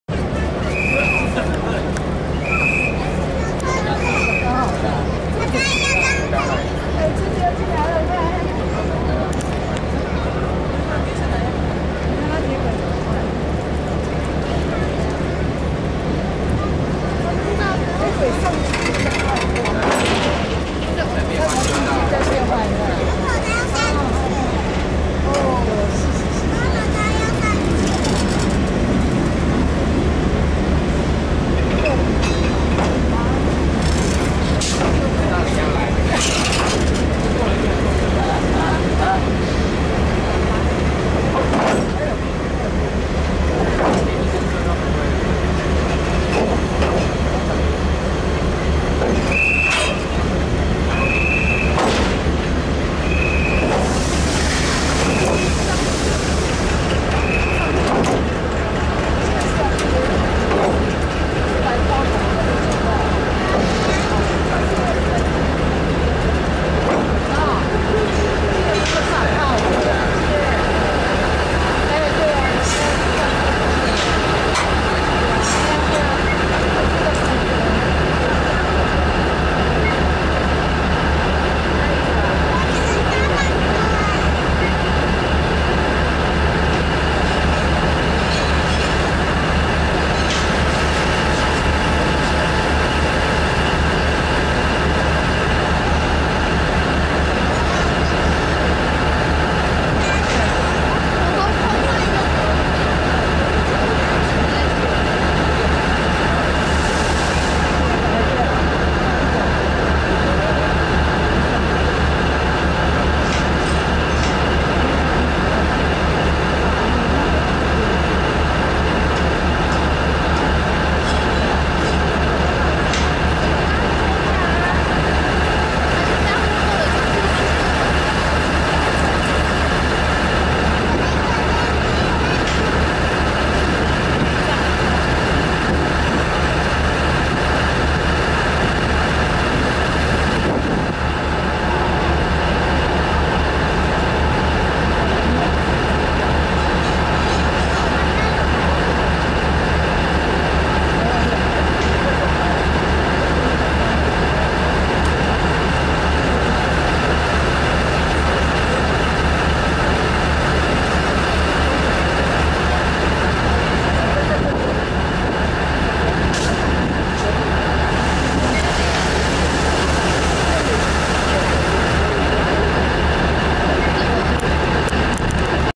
GE E300 and DHL Locomotive/彰化扇形車庫 E300 DHL 100 Part 1
TRA E316 E334 and DHL 109 Shunting at round house locomotive shop.